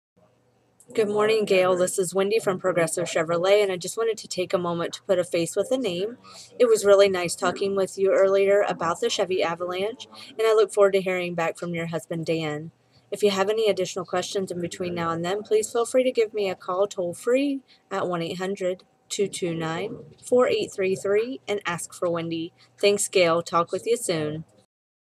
Chevy Avalanche for Gale of Massillon
Wendy putting a face to a name for Gale